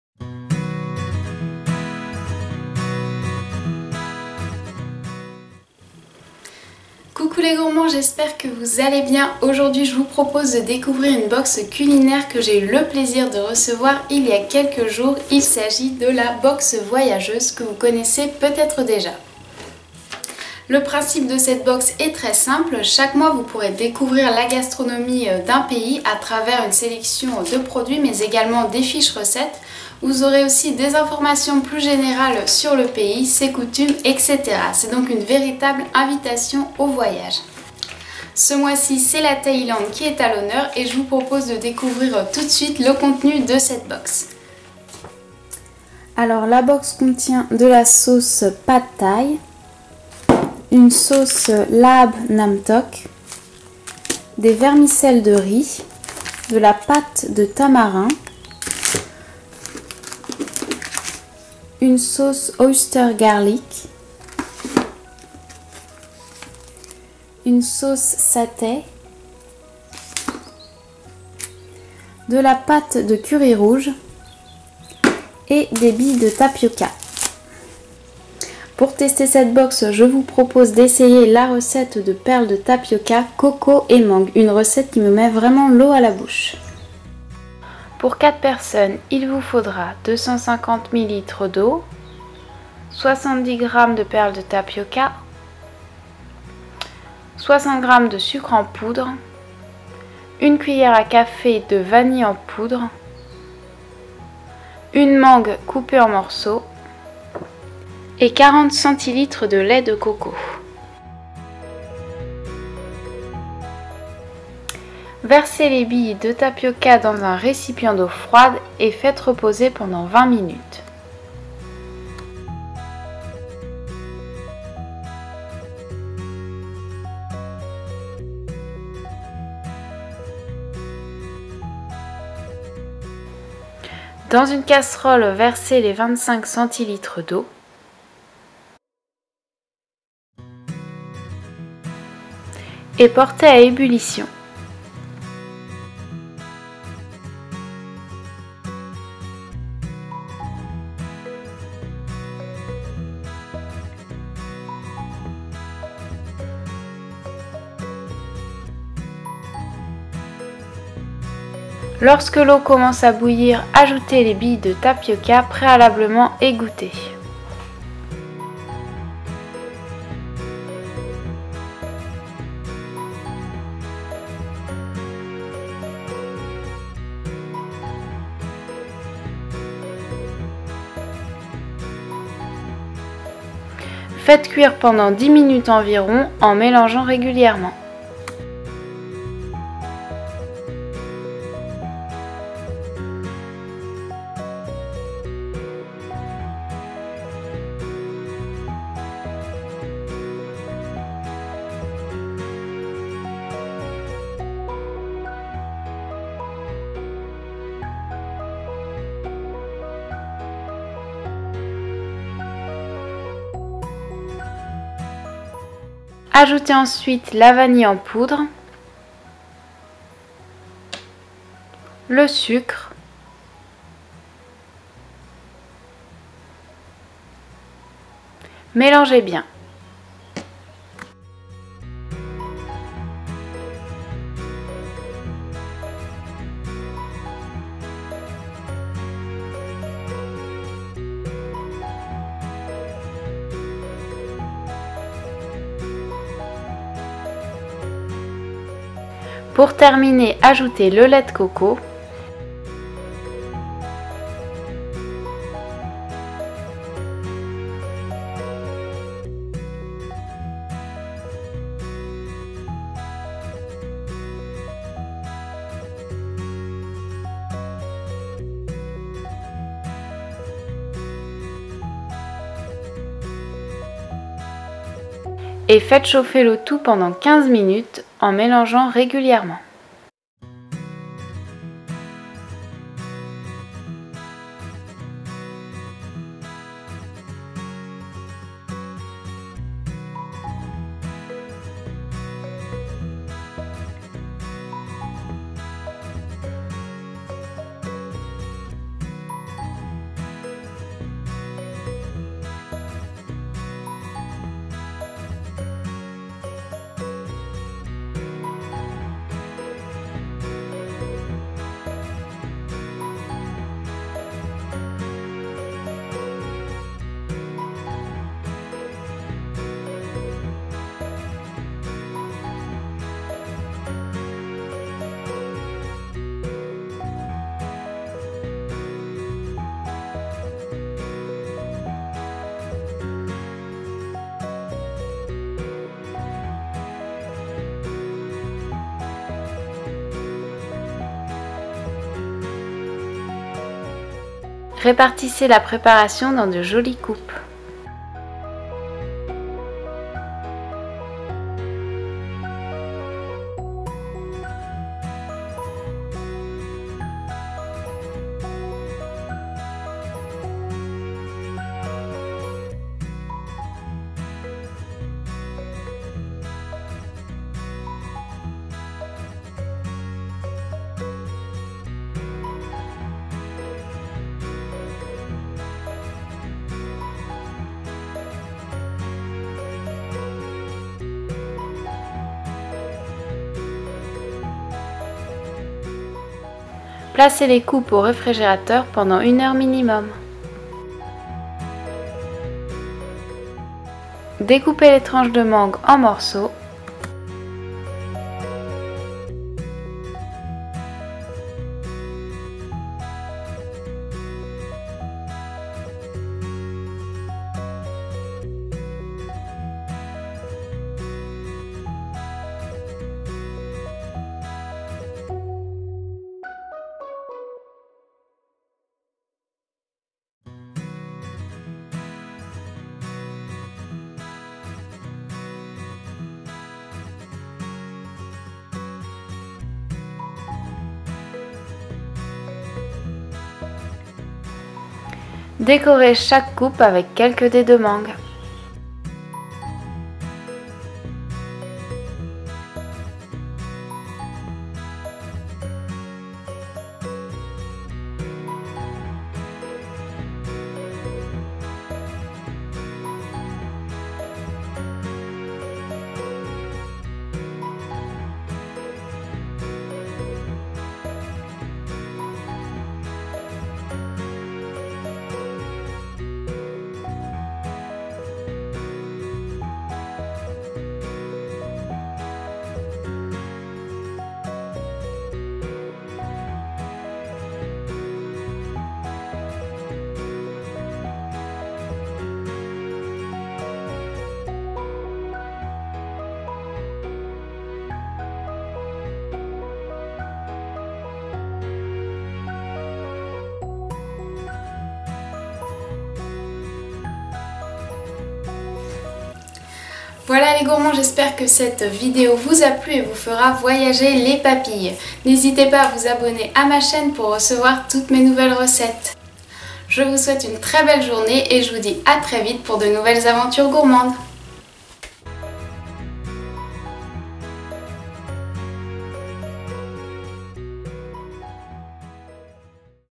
❥ Perles de tapioca coco et mangue
Direction la Thaïlande avec cette recette de perles de tapioca coco et mangue, réalisée avec la "Box Voyageuse".

❤ Instagram : Le Pays des Gourmandises